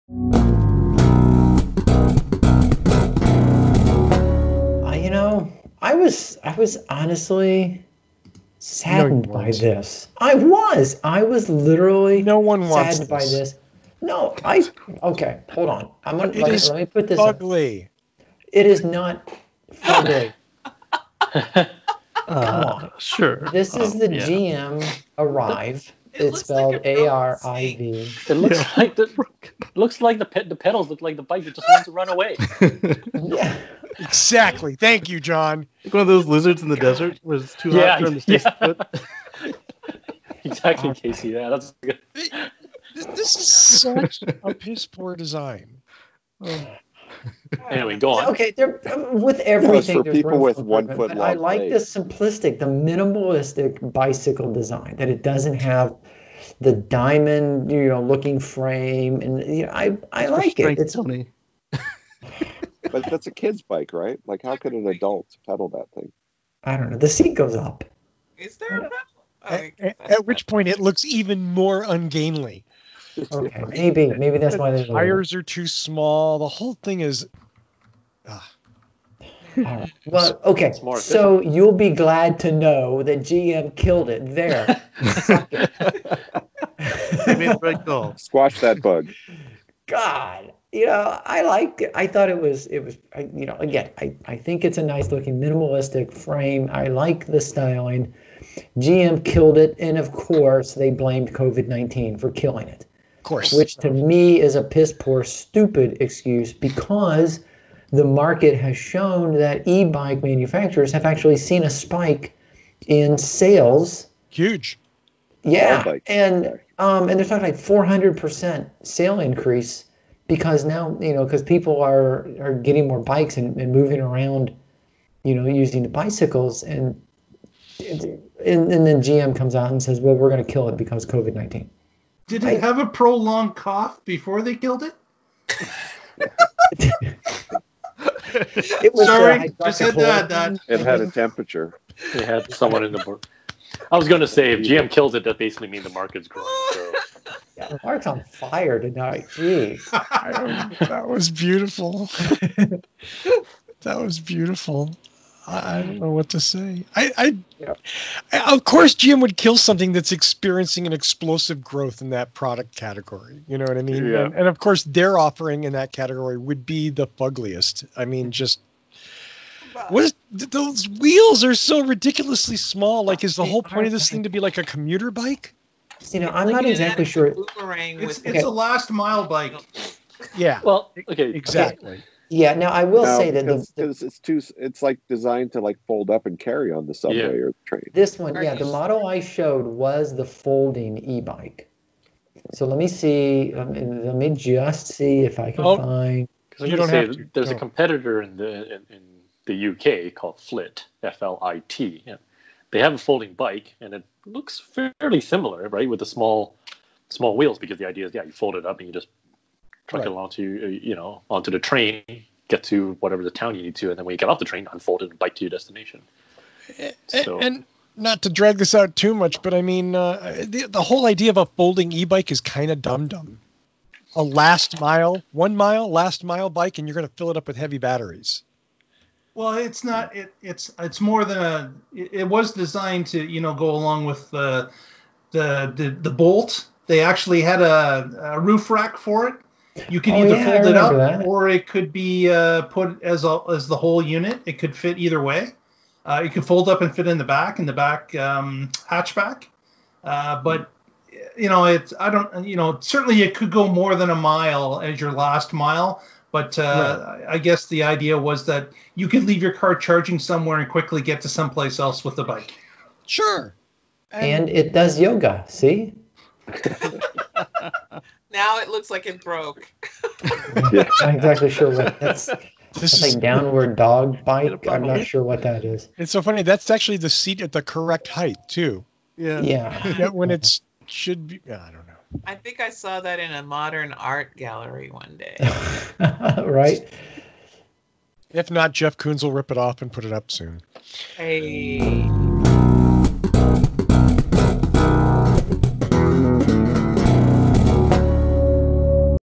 WDU Short - 367.5 - GM kills the Ariv
GM had created an e-Bike called the Ariv (pronounced 'arrive').  They had a standard version and a folding version.  Sure it looks a little quirky, but in a time when e-bikes are seeing a huge uptick in sales, GM kills theirs.